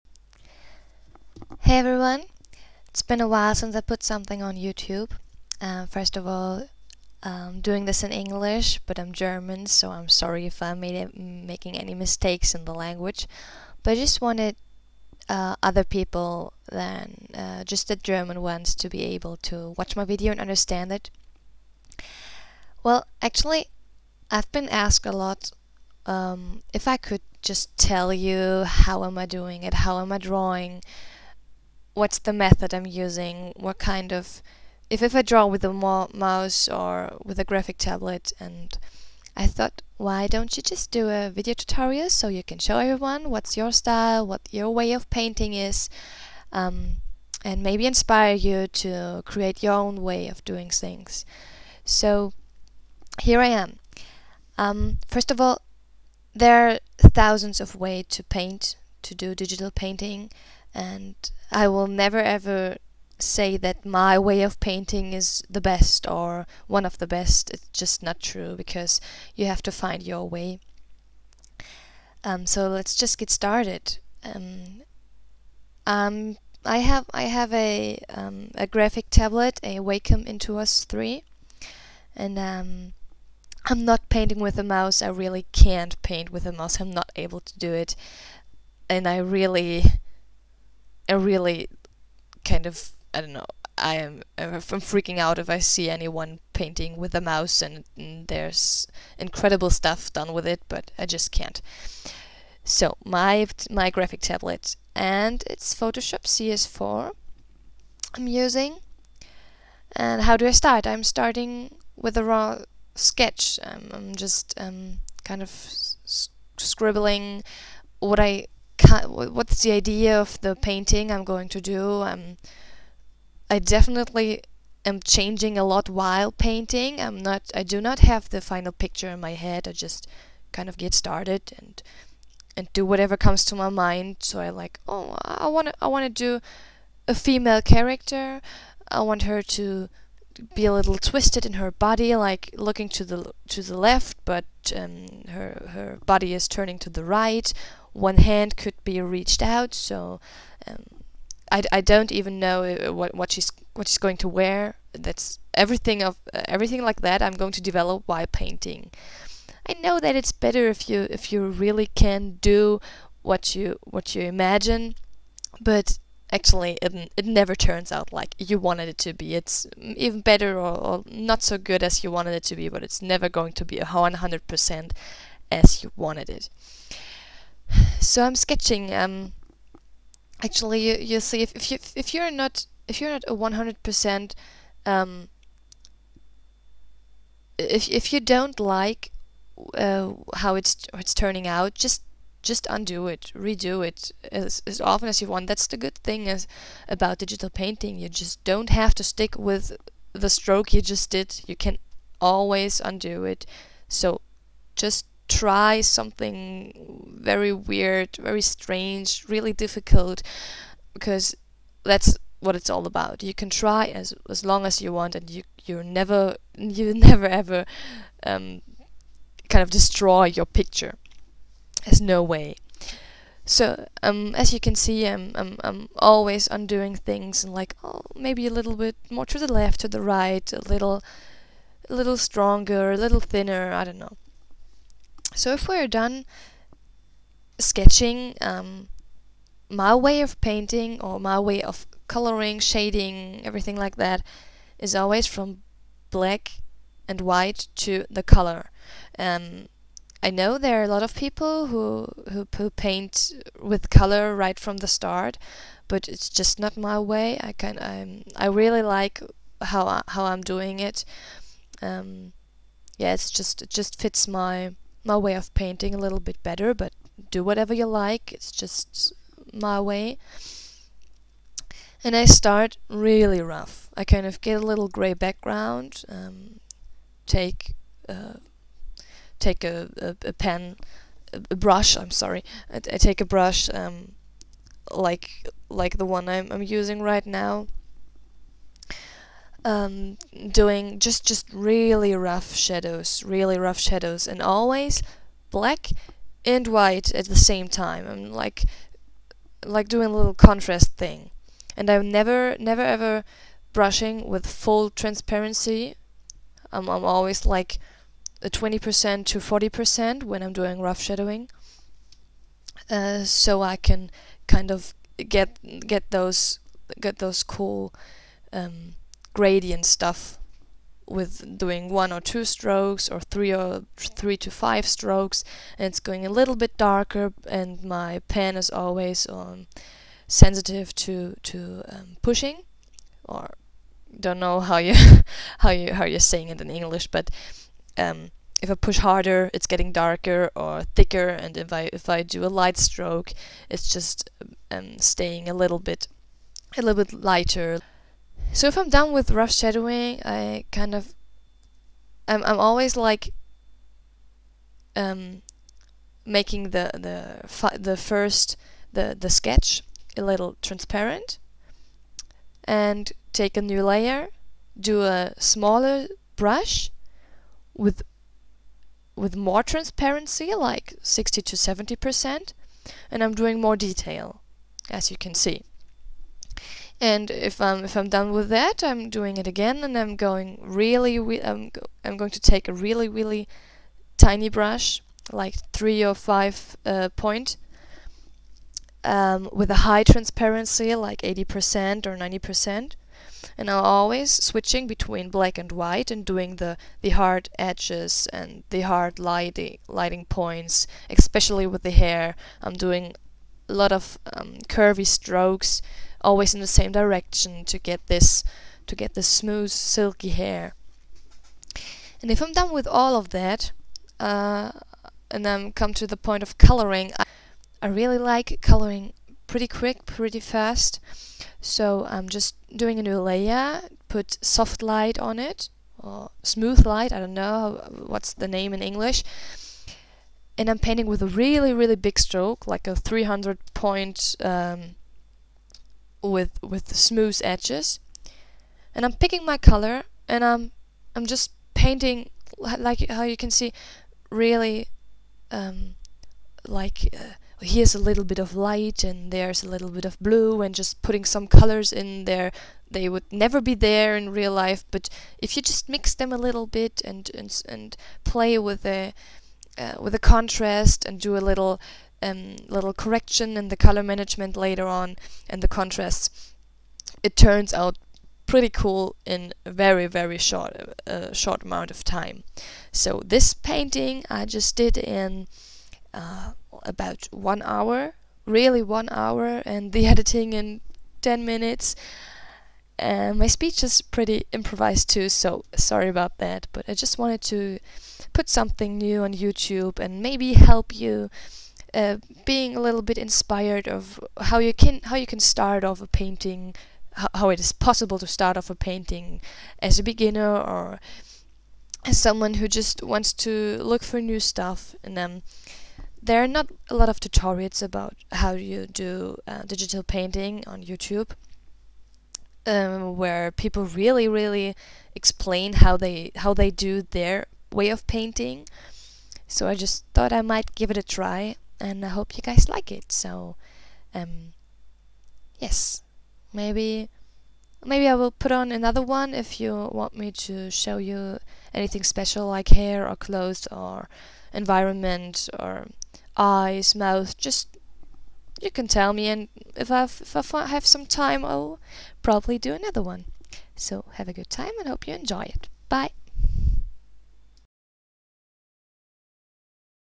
Digital Painting Tutorial - My way of Drawing
Short tutorial about my way of painting with photoshop.
there are thousands of ways to paint in photoshop -  this is just my own style :)

it's been requested a lot that i tell/show you how i am doing it ! so i hope u guys enjoy it and maybe learn something :)

this picture has been finished in only 1 hour -  just for the tutorial, so there's definitely not the amount of detail i would usually put in there :)

comments &  further requests are appreciated !

greetings
mimikri